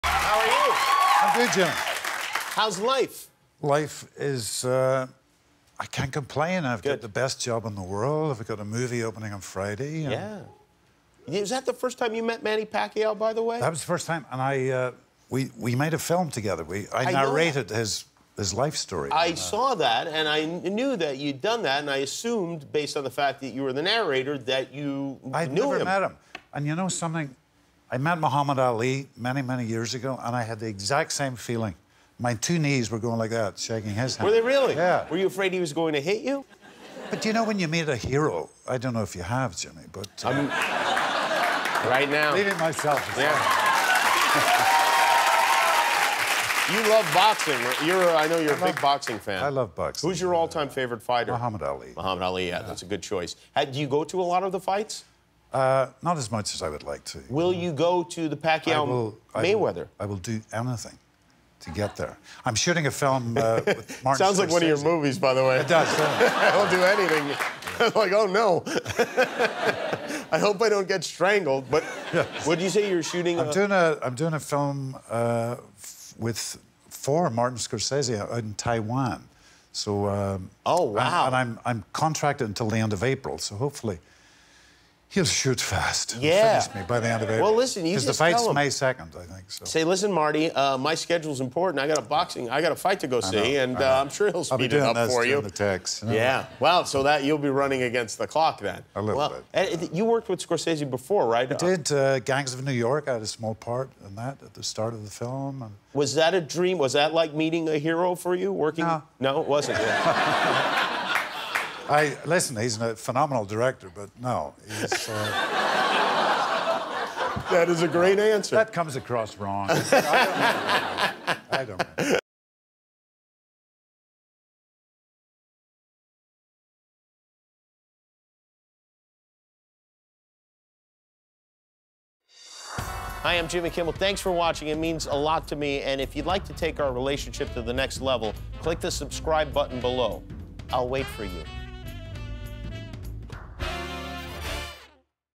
Liam Neeson on Boxing and Scorsese
Liam shares his love of boxing and talks about working with legendary director Martin Scorsese.